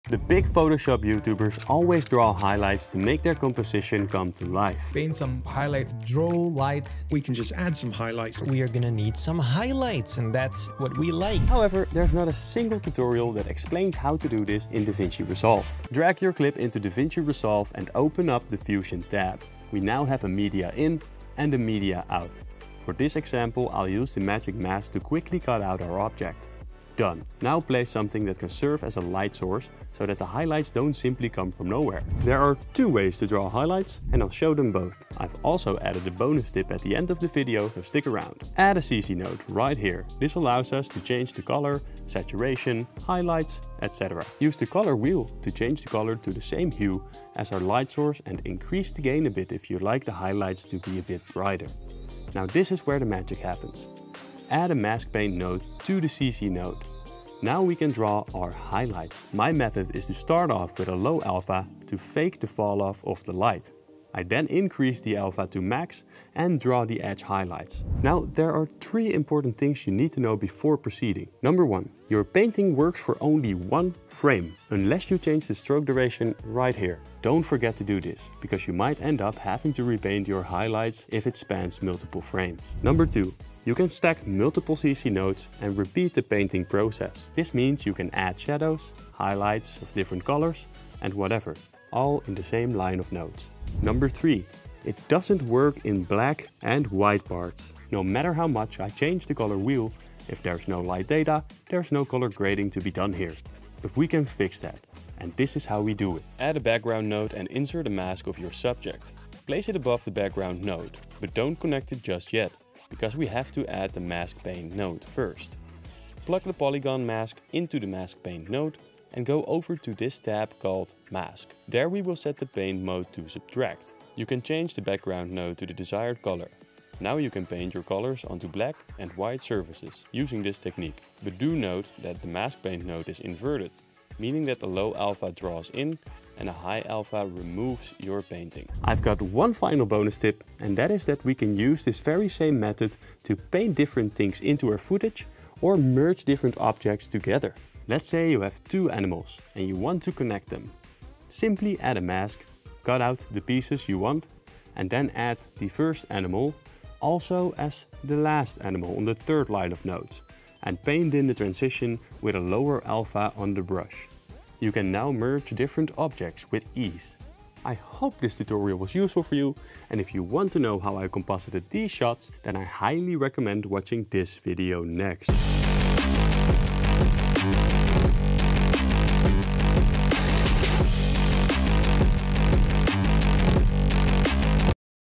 Draw Highlights in Davinci Resolve Just Like Photoshop
This is how you paint Highlights in Davinci Resolve. There are a lot of tutorials showing how to do this in Photoshop, but I've found literally zero when it comes to Davinci Resolve's Fusion. So I hope this tutorial fills the void.

Also if you have any tips, they're always welcome.

00:00 Intro
00:15 Set up Fusion
00:32 Highlights
02:17 BONUSTIP